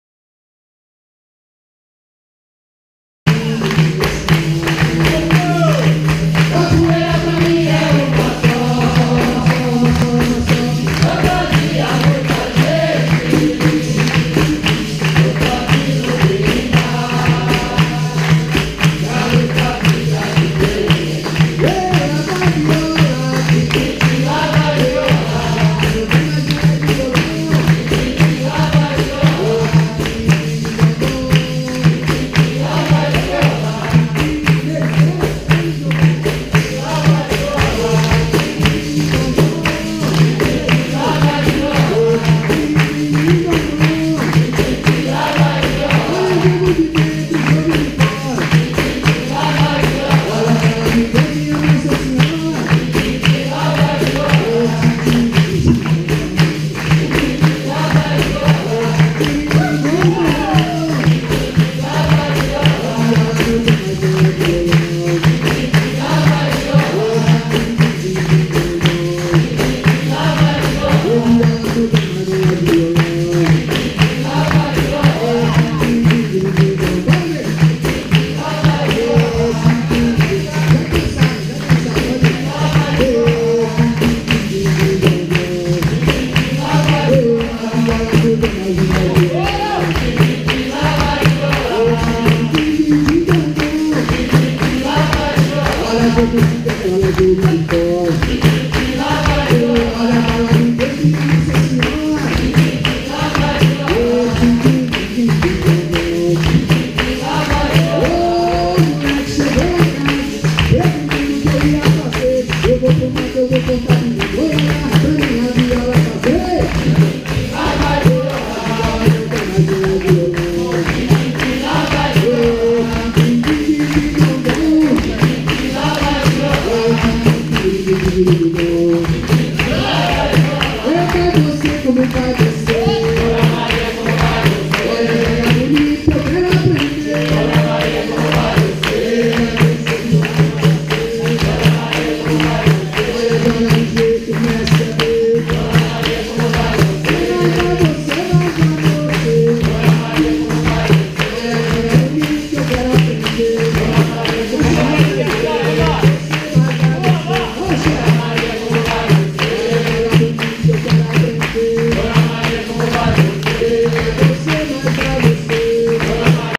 Força Rara Capoeira Batizado 2017 - Lenexa, KS - Open Roda 1
Contramestre Molejo, Contramestre Lobinho, Contramestre Fenix, Instrutor Negro